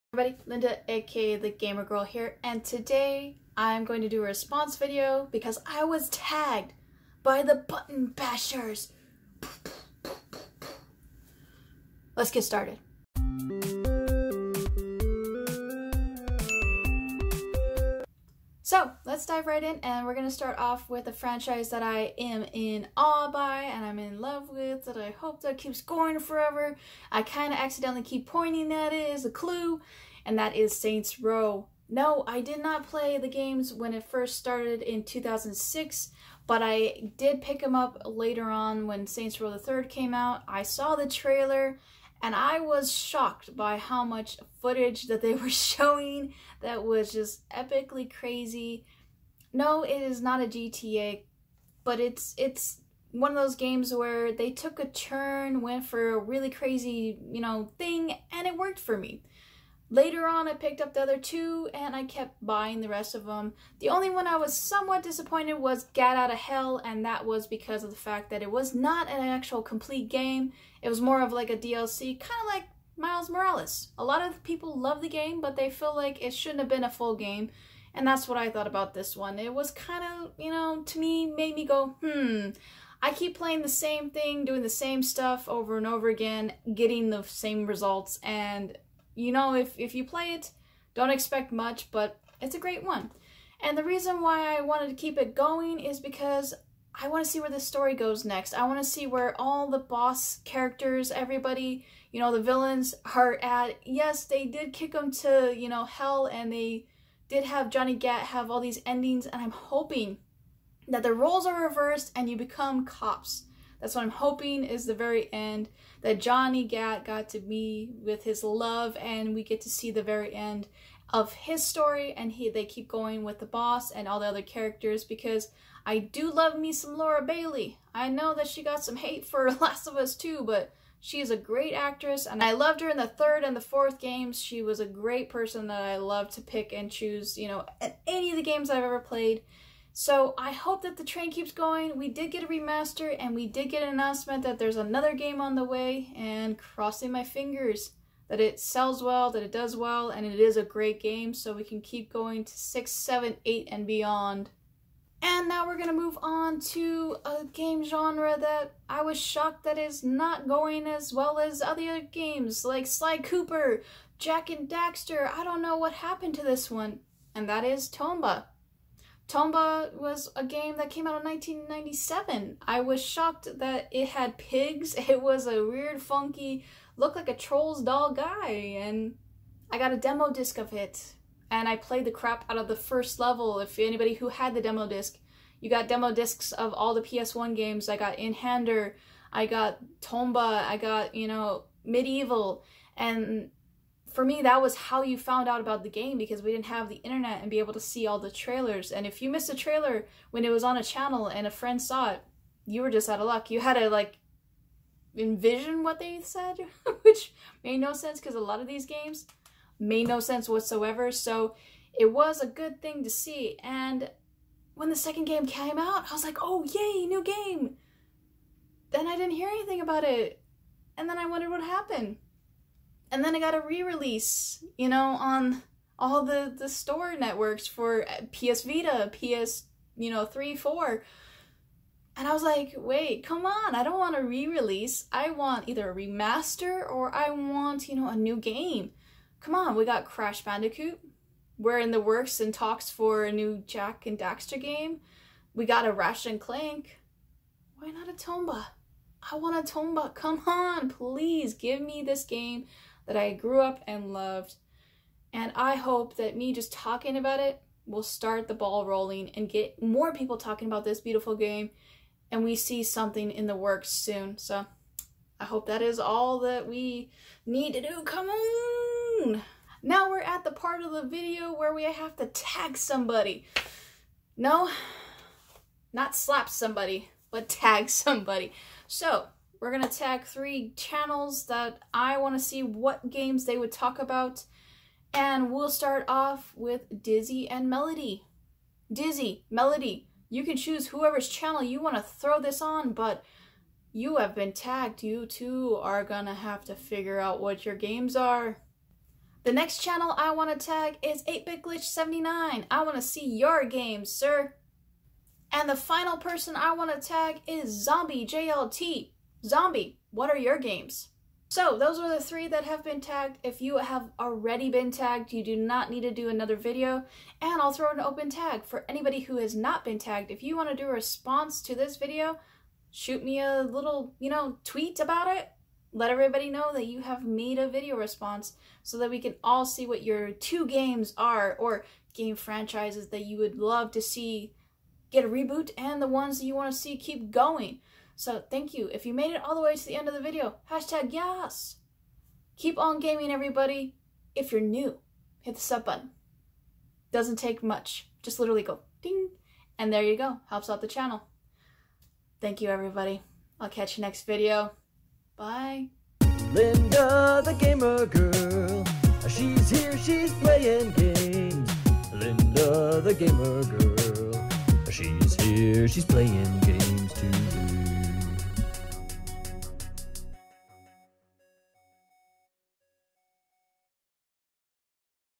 One to Stay and One to Reboot | Tagged by The Button Bashers
Here is my response to the Button Bashers.

Original Song Remix made by Peter Batemon
Peter Batemon's Channels: